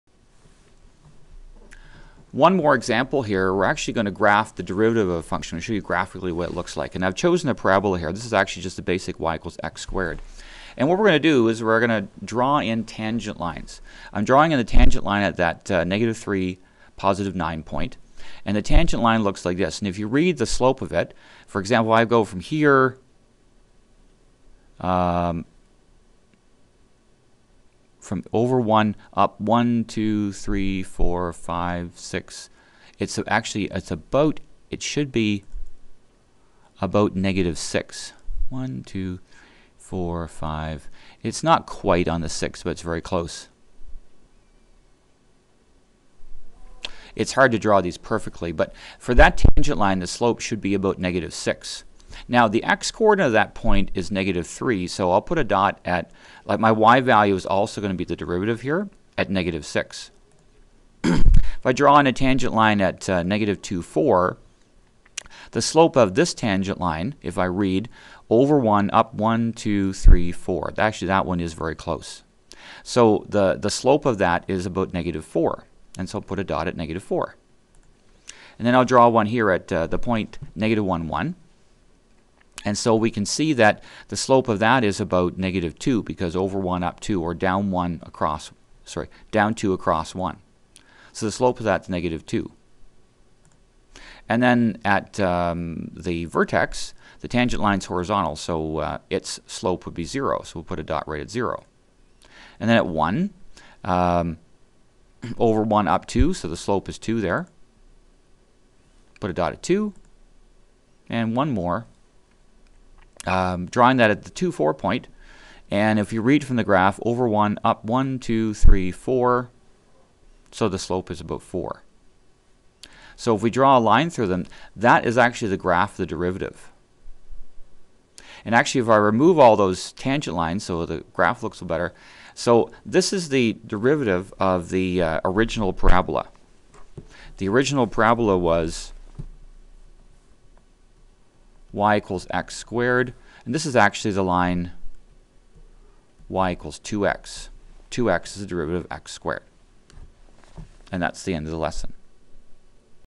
Introduction to the Derivative Part 3
This lesson discusses the concept of the derivative, including the "first principles definition". This is the third part of a three part lesson. This lesson was created for the Calculus and Vectors (MCV4U) course in the province of Ontario, Canada.